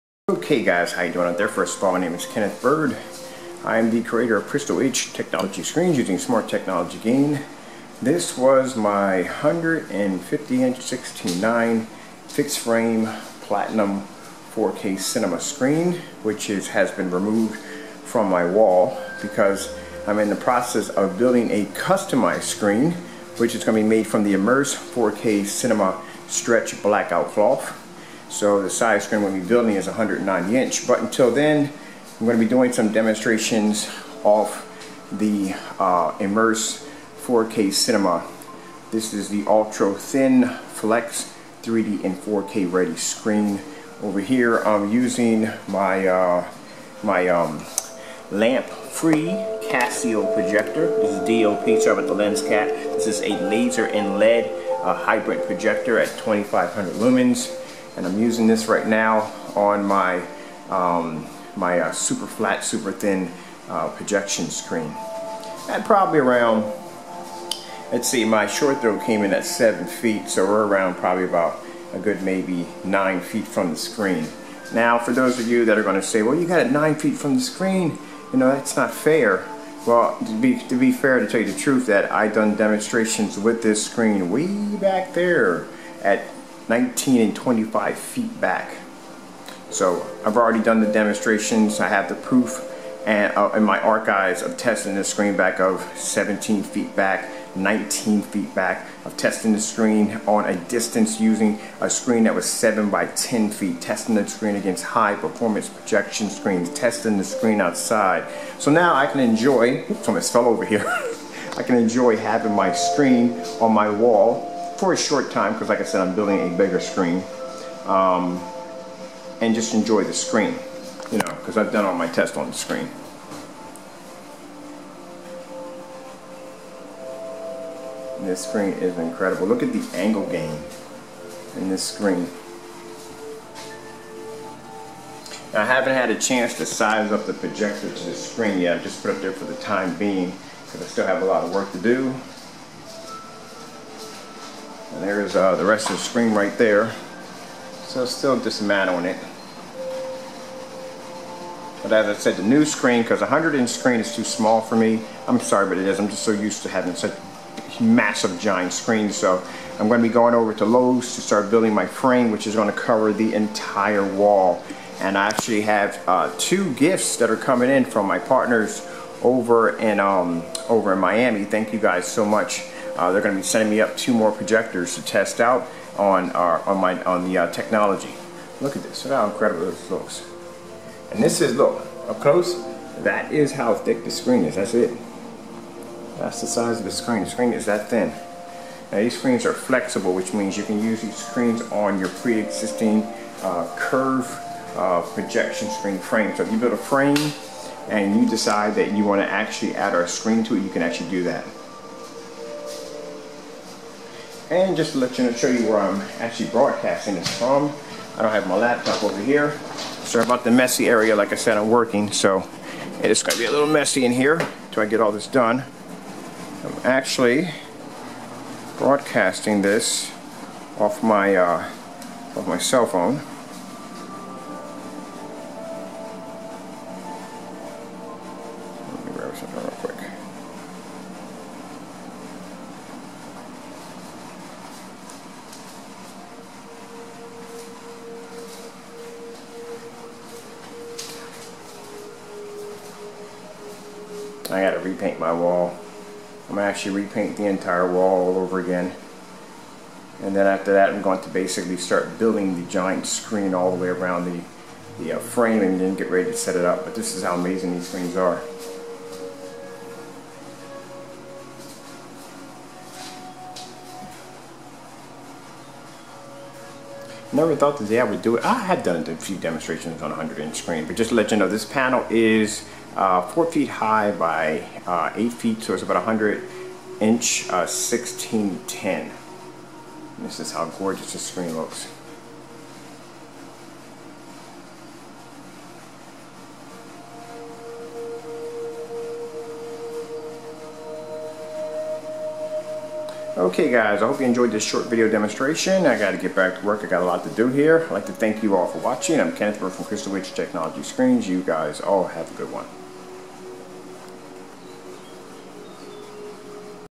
MY NEW IMMERSE 4K CINEMA SCREEN AND NEW IMMERSE FIX FRAME SCREEN PROJECT
NAME: Immerse 4k cinema
PROJECTION SCREEN TYPE: Panel projection screen
SCREEN COLOR: Titanium Platinum.
GAIN: Smart technology gain X4
ANLGE GAIN: 190
LUMEN COUNT: 2000 and up.
3D: Yes.
4K: Yes 4K  ready.
PROJECTORS: 4K, 1080P, LASER, DLP, LED, LCD, Short and long throw projectors.
SCREEN SIZES: 160" Screen size: 139” x 79”
RATIO: 16:9
For Indoor only
Viewing picture enhancement capability has the ability to produce amazing outstanding  1080p or 4K colors, Deep black contrast levels and pure snow whites.
Immerse yourself in the ULTIMATE home theater screen
Available overseas